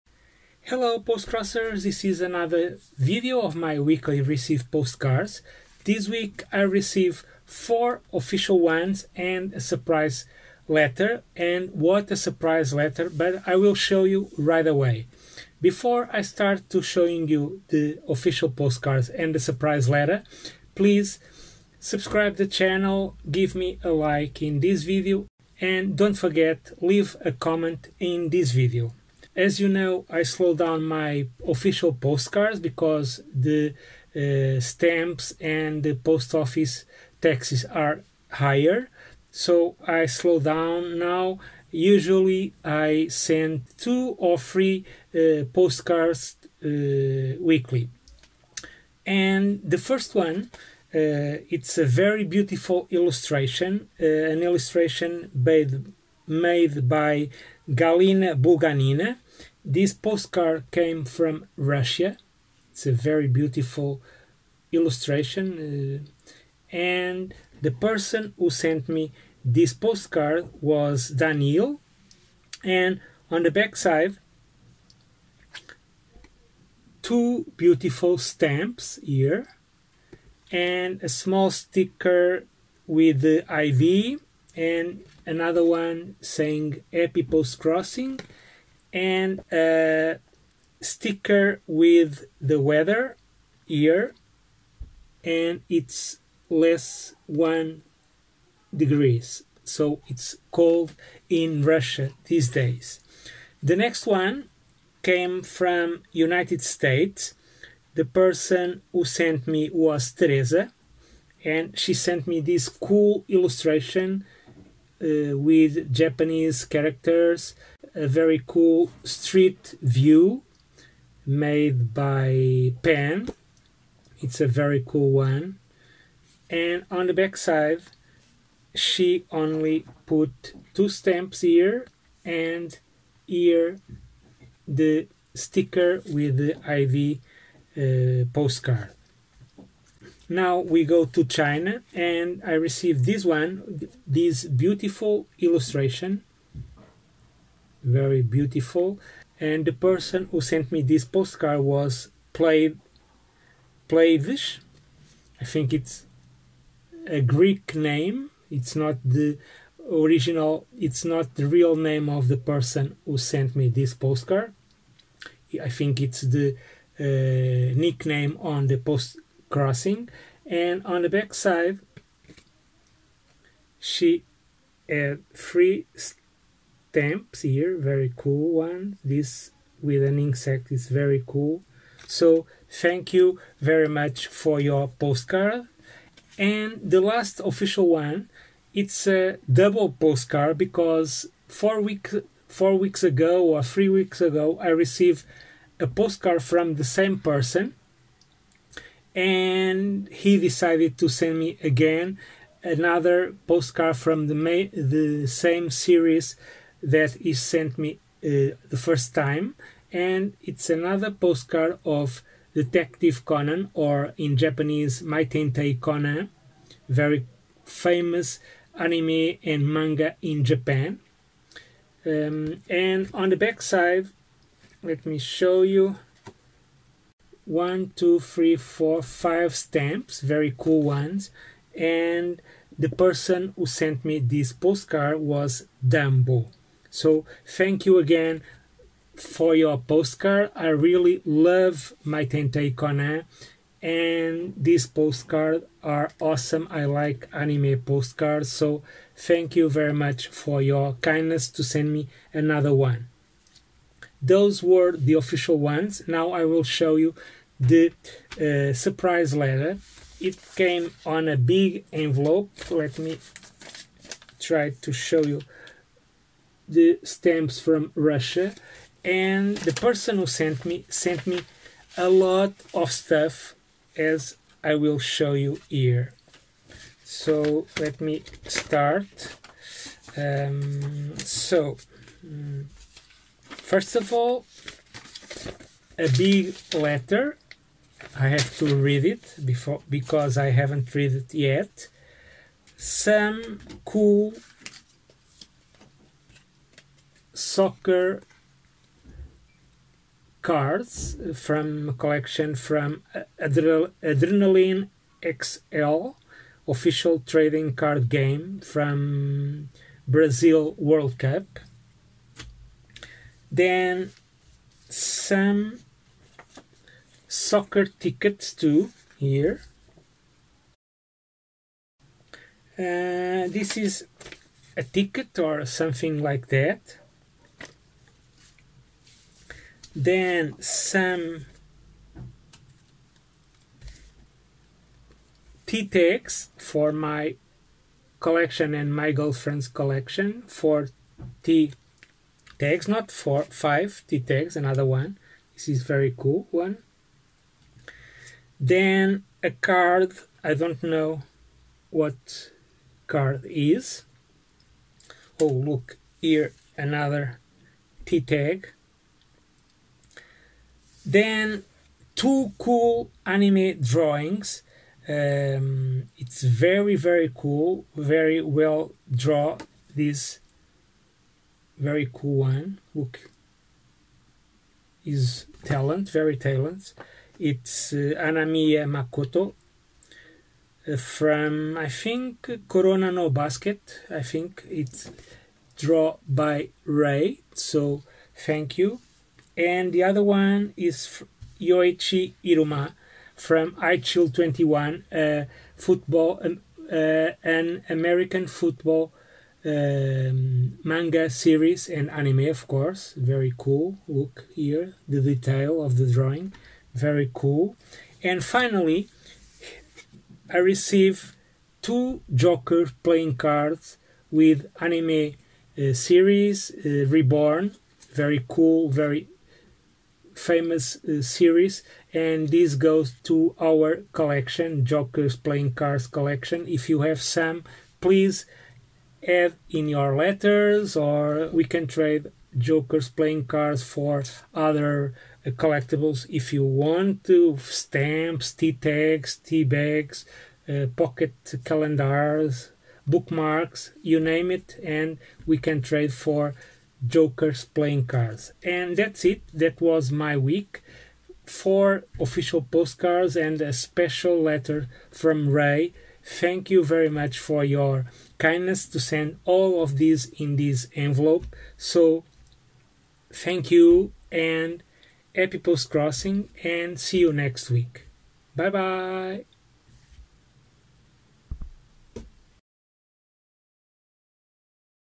POSTCROSSING || WEEK 206
These week I received 4 oficial postcards from:

- 1 postcard from Russia (Oficial Postcrossing)
- 1 postcard from United States (Oficial Postcrossing)
- 2 postcards from China (Oficial Postcrossing)

- 1 letter from Russia (Surprise Letter)